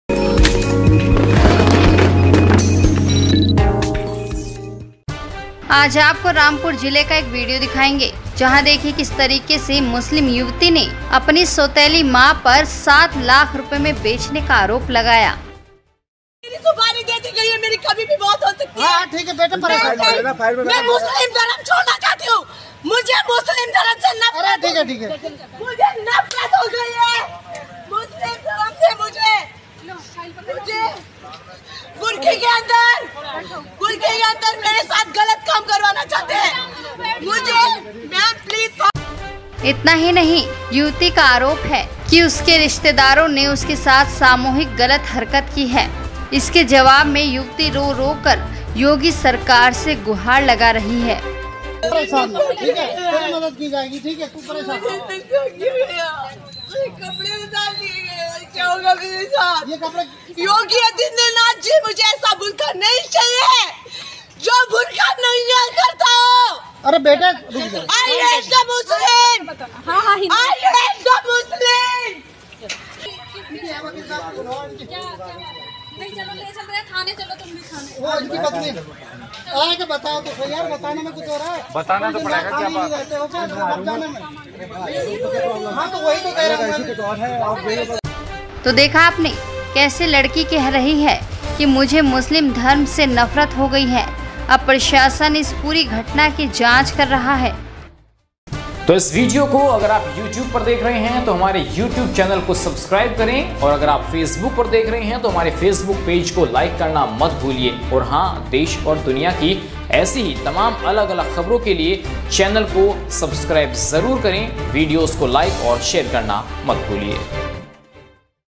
Daughter को Mother ने ₹7 लाख रुपए में बेच दिया तो Police से बोली लड़की बुर्के और Muslim होने से नफरत
Daughter को Mother ने ₹7 लाख रुपए में बेच दिया तो Police से बोली लड़की बुर्के और Muslim होने से नफरत ! Watch Out Full Video And News !